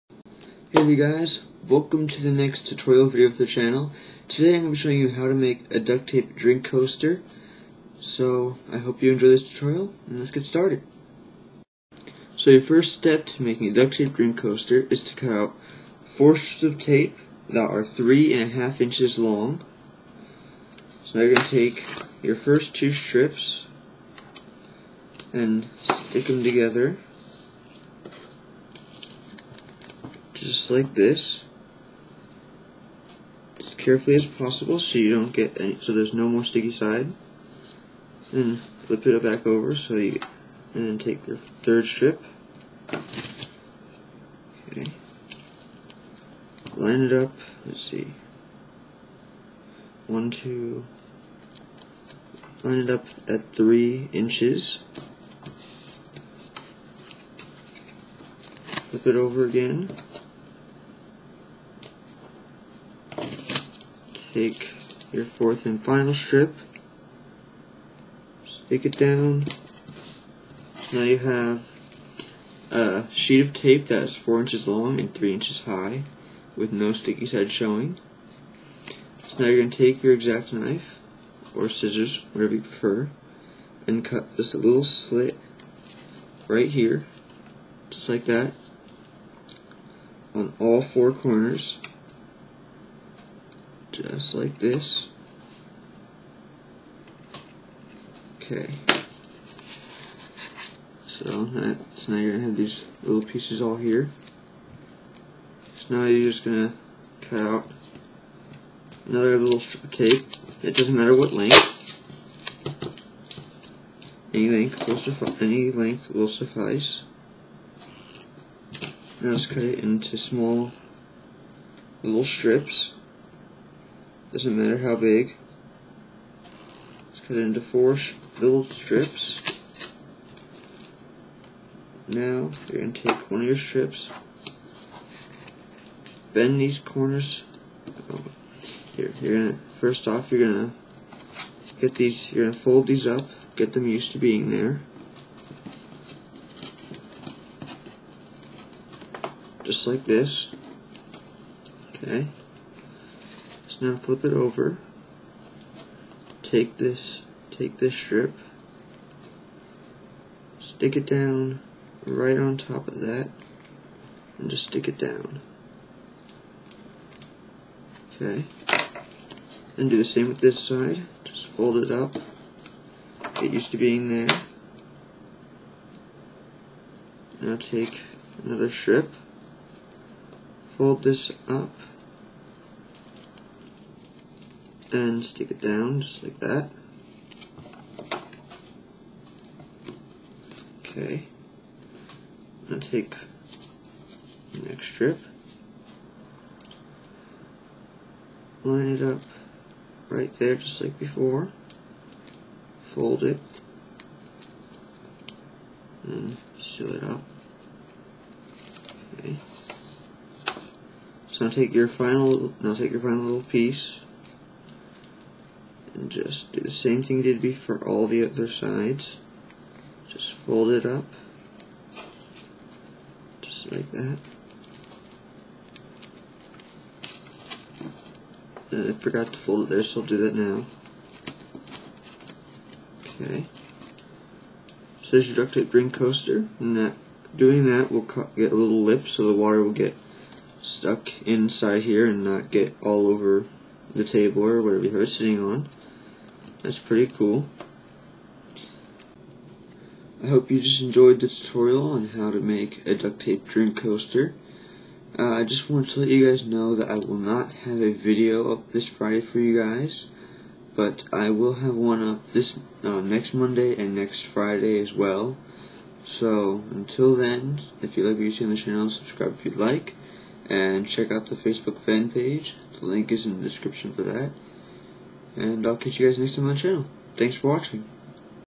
How to make a duct tape drink coaster
hey there guys welcome to the next how to video for the channel so i hope you enjoy. I am sorry for the late upload.

And for awesome step by step tutorials visit a channel called ducttapestuff if you'd like here' the